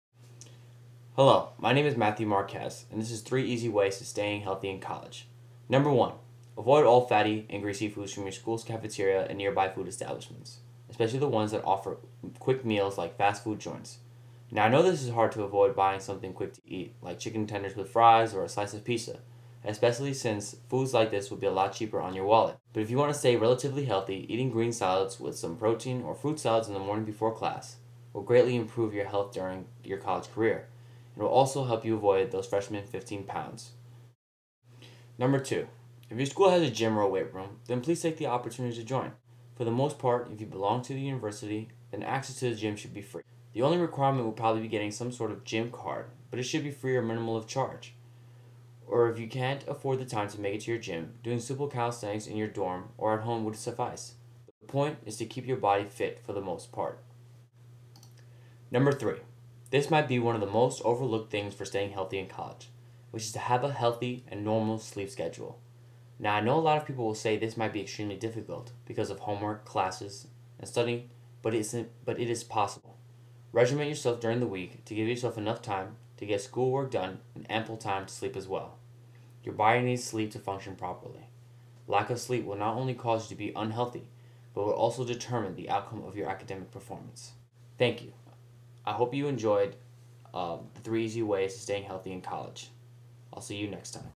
How To Stay Healthy In College
- Worried about the freshman 15? Rest assured there are simple and effective ways to change bad habits and learn how to stay healthy in college! Watch here to discover how!